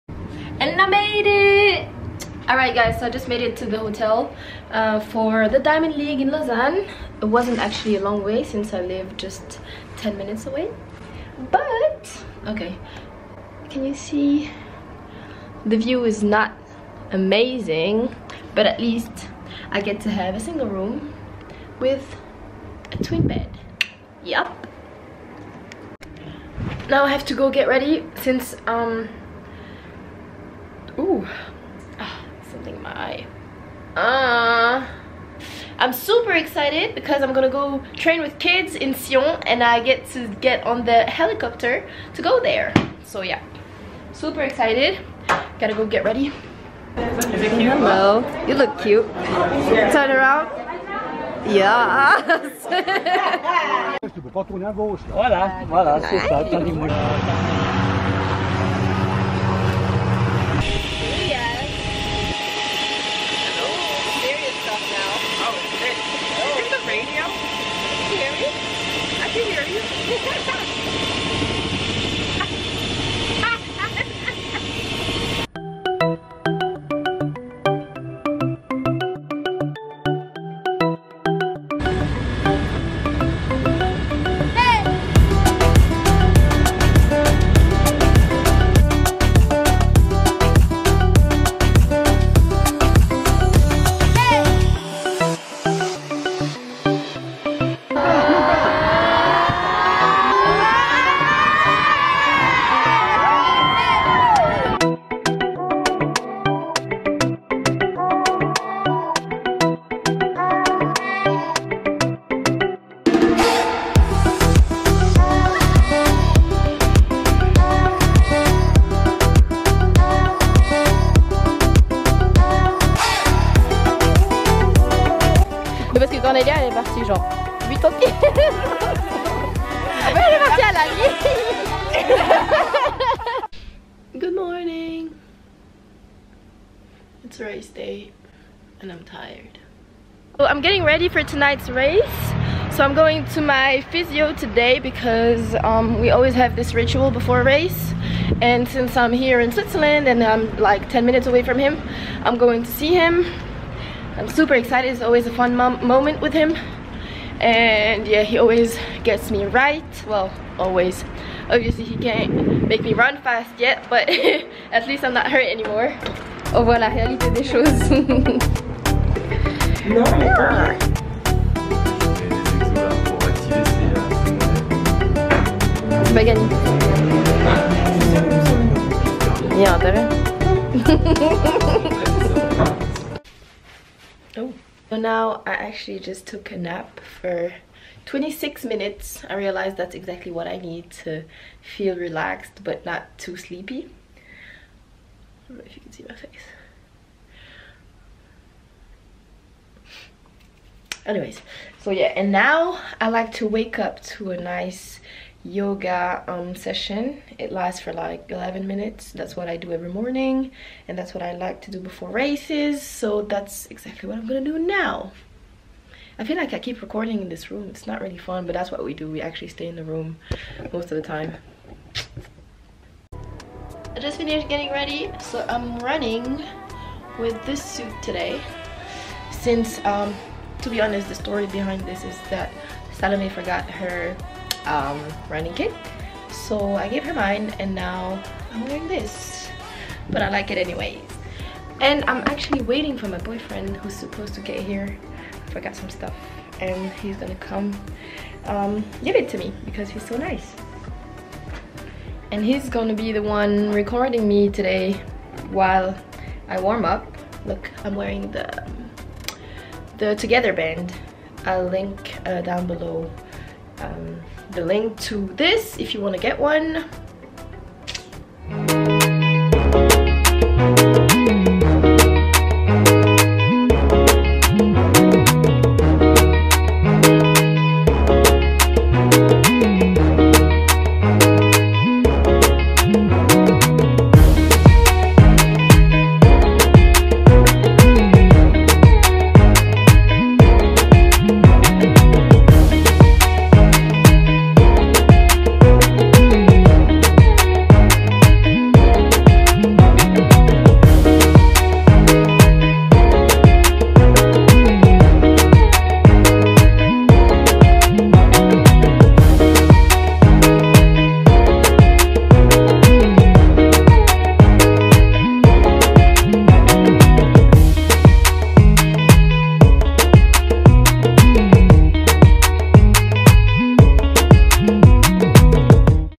LAUSANNE DIAMOND LEAGUE VLOG *in English*
*LAUSANNE DIAMOND LEAGUE*

So Im not having the season of my life but the organisation of the Diamond League still blessed me with a lane at my favorite race !!

I obviously took the opportunity and ran a season's best but Im far from where I was last year...

Also you see in the video a link to the Together band which you can buy to be part of the global movement
"TOGETHERBAND unites us as a global community, sharing commitment to all of the 17 UN Global Goals. Our aim is to bring together one billion active citizens to achieve these Goals."

Anyways I hope you enjoy the video :-)

Sarah
xx